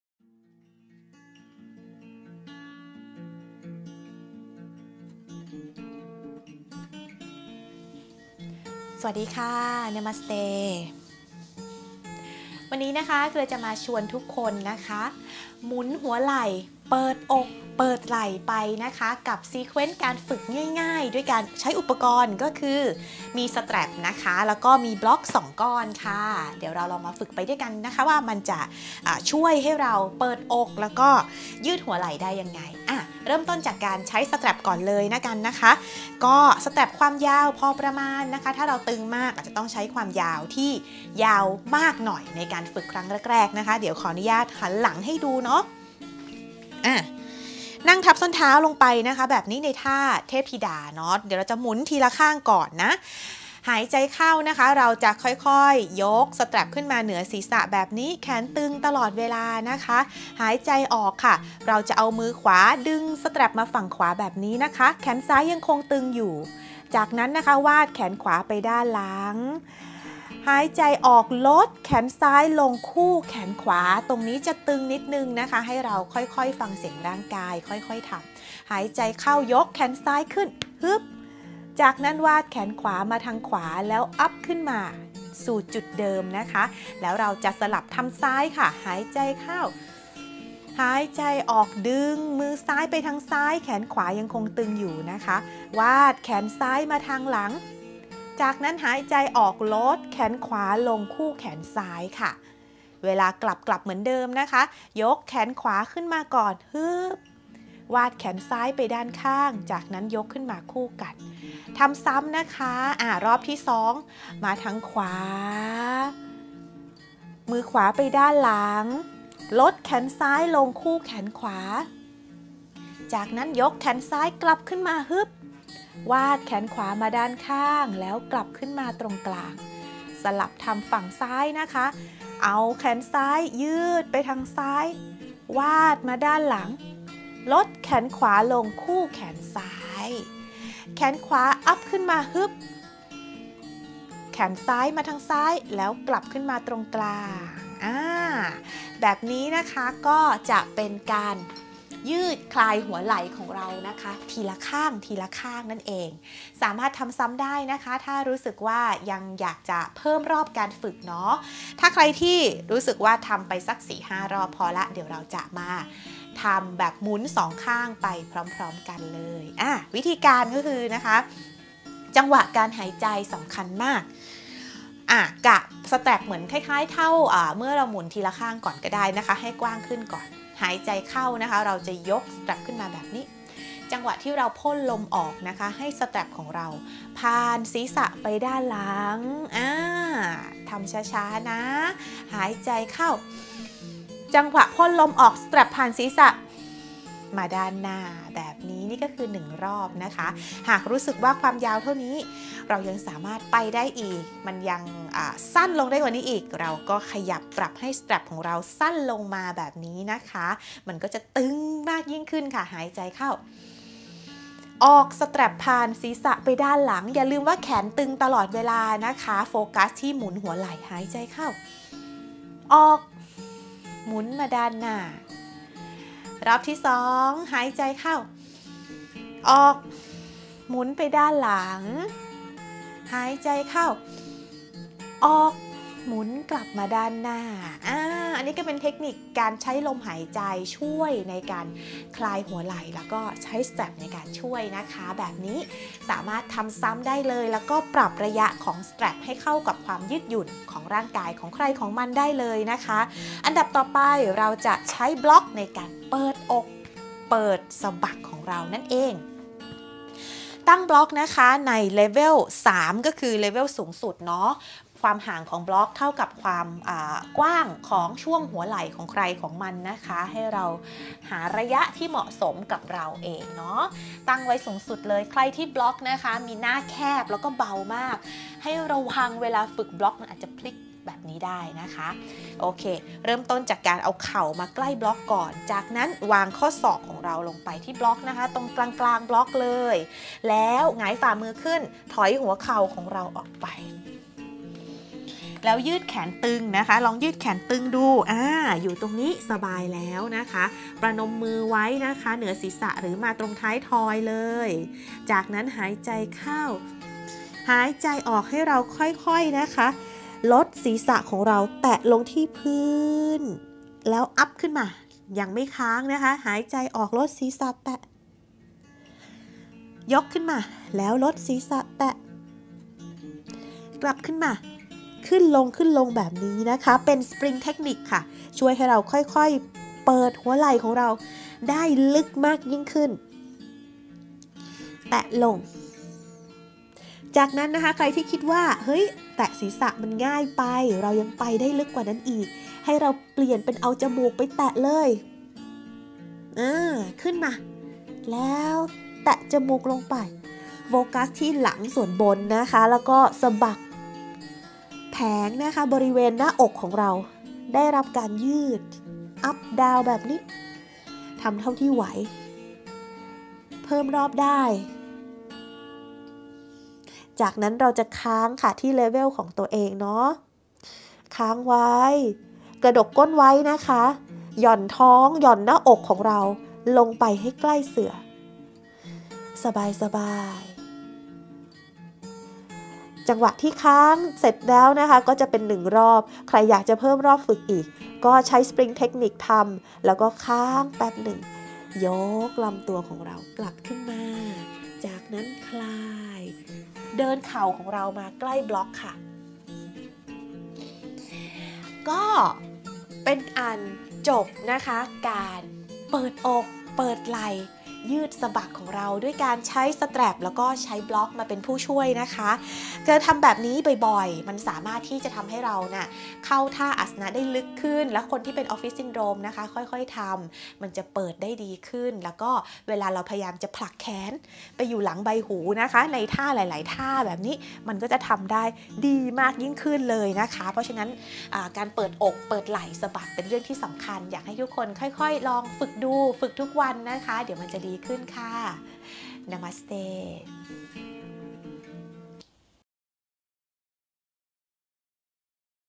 โยคะแก้ไหล่ติด ออฟฟิศซินโดรม
ไหล่ติด ไหล่ตึง ออฟฟิศซินโดรม เชิญทางนี้ค่ะ ชุดหมุนหัวไหล่ เปิดอกชุดนี้ดีมากๆ ฝึกตามเลเวลของตัวเองบ่อยๆจะช่วยป้องกันการเกิดออฟฟิศซินโดรมได้ด้วยนะคะ ถ้าไม่มีสแตรป (เชือกโยคะ) สามารถใช้ผ้าขนหนูหรือผ้าพันคอได้ค่ะ